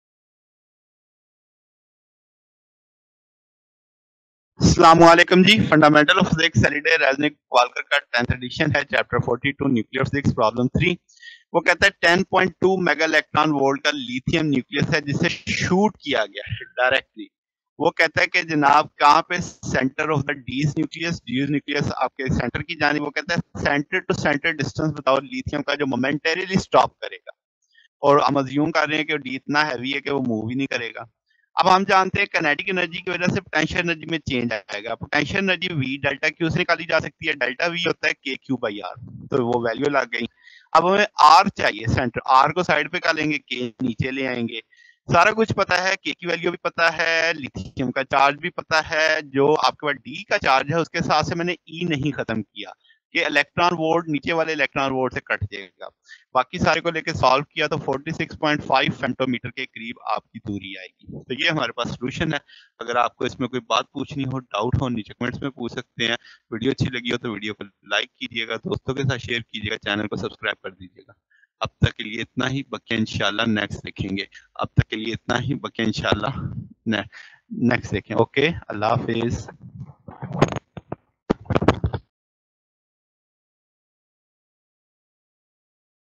Halliday resnick chapter 42 problem 3 solution | Fundamentals of physics 10e solutions
A 10.2 MeV Li nucleus is shot directly at the center of a Ds nucleus. At what center-to-center distance does the Li momentarily stop, assuming the Ds does not move?

resnick halliday
halliday physics